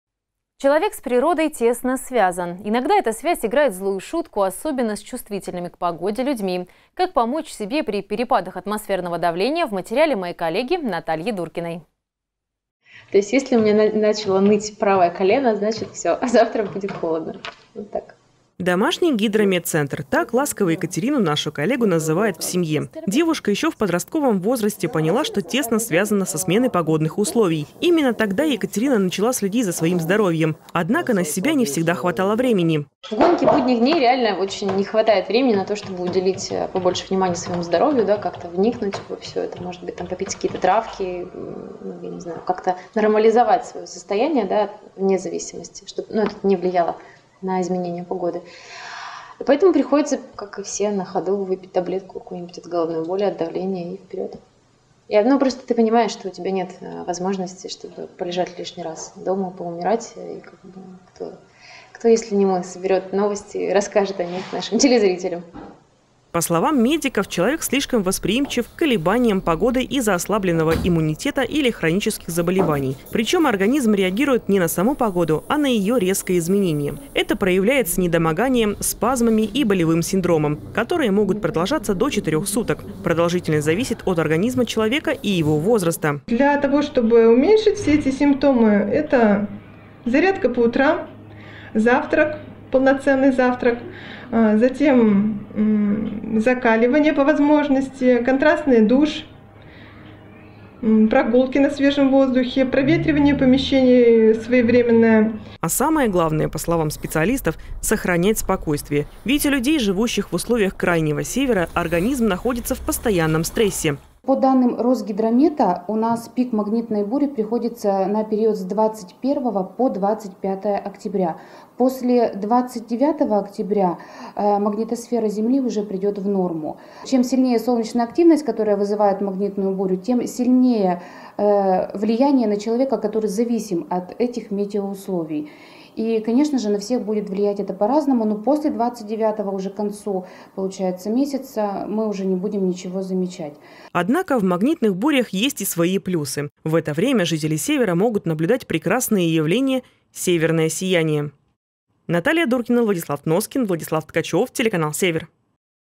МЕТЕОЗАВИСИМЫЕ ЛЮДИ
Человек с природой тесно связан. Иногда эта связь играет злую шутку, особенно с чувствительными к погоде людьми. Как помочь себе при перепадах атмосферного давления в материале Натальи Дуркиной.